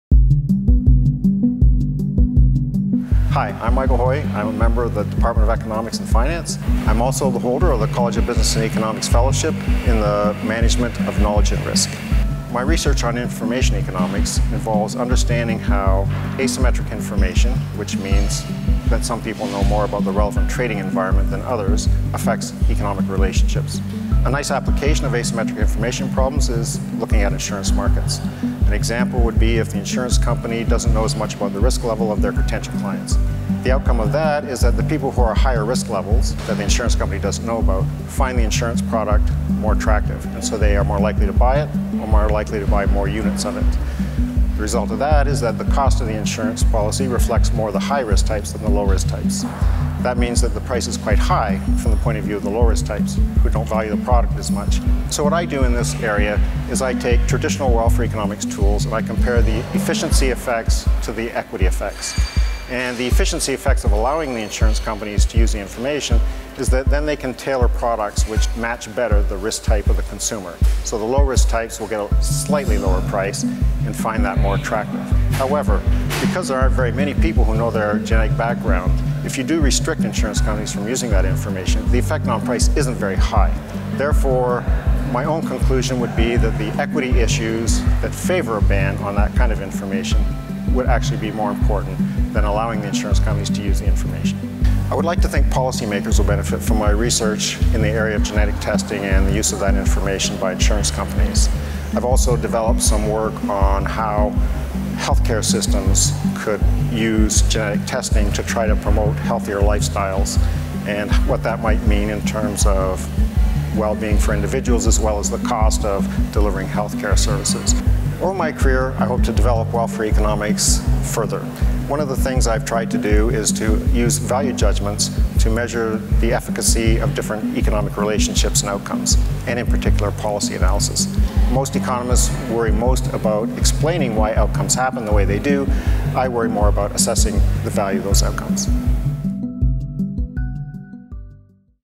Mike Hoy, College of Business and Economics Fellow in Knowledge and Risk
Mike Hoy discusses his research on insurance and risk as it pertains to health and life insurance.

Video transcript
Hi I’m Michael Hoy, I’m a member of the Department of Economics and Finance, I’m also the holder of the College of Business and Economics Fellowship in the Management of Knowledge and Risk. My research on information economics involves understanding how asymmetric information, which means that some people know more about the relevant trading environment than others affects economic relationships. A nice application of asymmetric information problems is looking at insurance markets. An example would be if the insurance company doesn’t know as much about the risk level of their potential clients. The outcome of that is that the people who are higher risk levels that the insurance company doesn’t know about find the insurance product more attractive so they are more likely to buy it or more likely to buy more units of it. The result of that is that the cost of the insurance policy reflects more the high-risk types than the low-risk types. That means that the price is quite high from the point of view of the low risk types who don’t value the product as much. So what I do in this area is I take traditional welfare for economics tools and compare the efficiency effects to the equity effects. And the efficiency effects of allowing the insurance companies to use the information is that then they can tailor products which match better the risk type of the consumer. So the low risk types will get a slightly lower price and find that more attractive. However because there aren’t very many people who know their genetic background if you do restrict insurance companies from using that information the affect on price isn’t very high therefore my own conclusion would be that the equity issues that favor a ban on that kind of information would actually be more important than allowing the insurance companies to use the information. I would like to think policy makers would benefit from my research in the area of genetic testing and the use of that information by insurance companies. I’ve also developed some work on how healthcare systems could use genetic testing to try to promote healthier lifestyles and what that might mean in terms of well being for individuals as well as the cost of delivering healthcare services. Over my career I hope to develop welfare economics further. One of the things I have tried to do is to use value judgments to measure the efficacy of different economic relationships and outcomes and in particular policy analysis. Most economists worry most about explaining why outcomes happen the way they do I worry more about assessing the value of those outcomes.